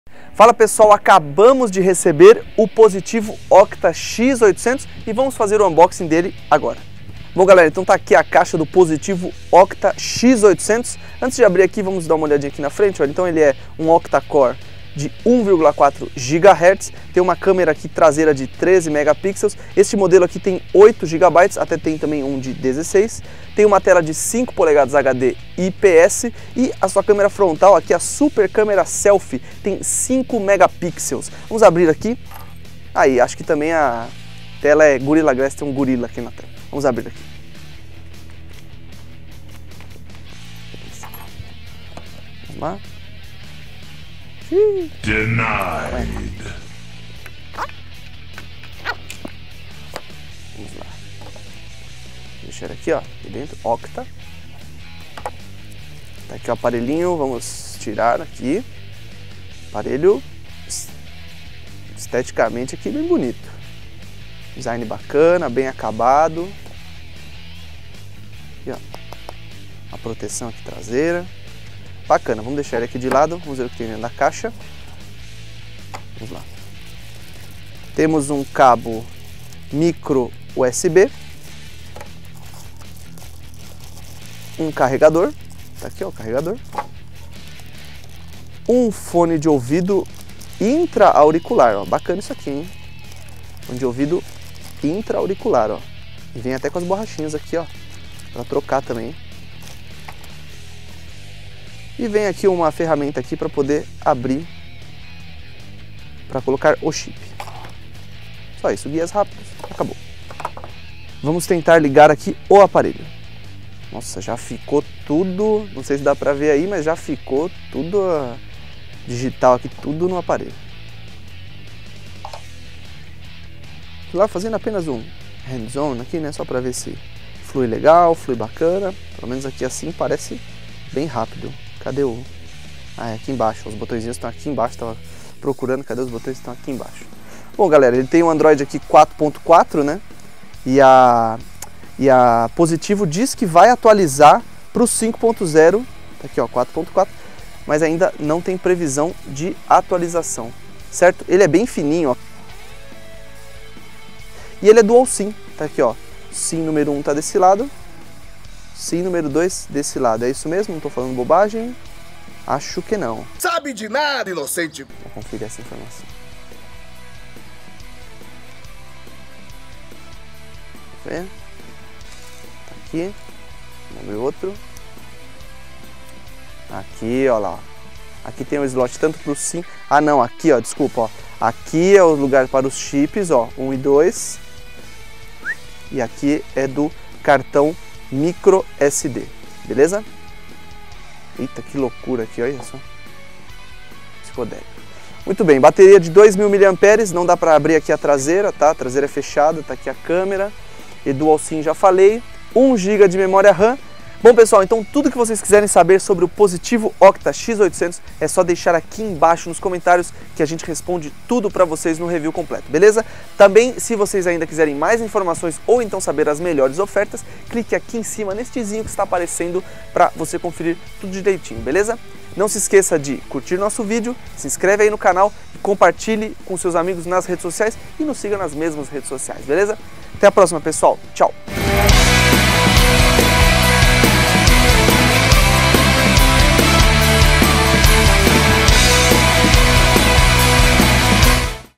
Positivo Octa X800 - Unboxing
A Positivo apresenta o OCTA X800, um smartphone com processador octa-core (8 núcleos), câmera frontal de 5MP para selfies e traseira de 13MP.

Confira o unboxing do aparelho, sua apresentação e o que vem dentro da caixa.

Este é o canal do Bondfaro. Reviews, análises, comparativos e notícias sobre os principais produtos e tecnologias.